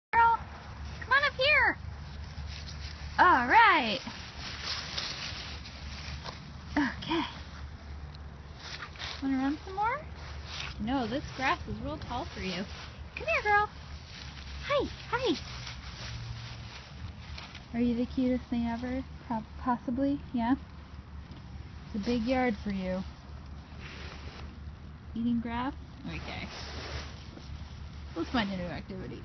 Ruby the Parti Yorkie
Here is a video of the available parti yorkie named Ruby. She is ready to come home anytime.